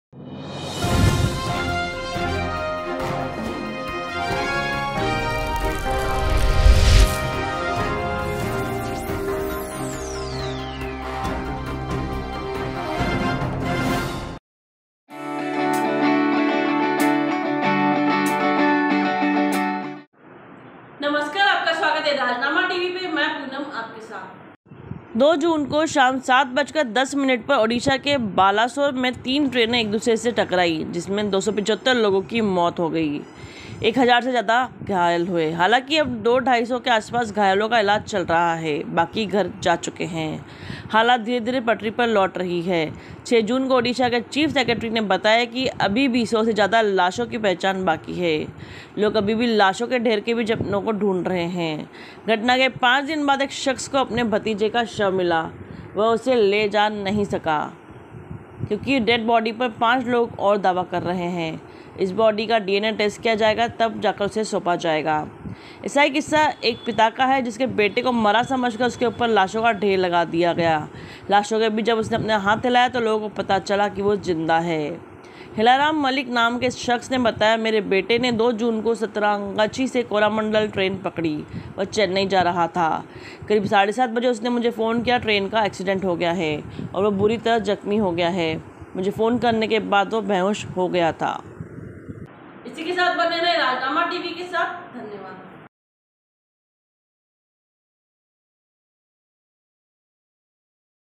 anchorpoonam ओडिशा ट्रेन हादसा, लाशों के बीच पड़ा रहा जिंदा शख्स
About this video:ओडिशा ट्रेन हादसा, लाशों के बीच पड़ा रहा जिंदा शख्स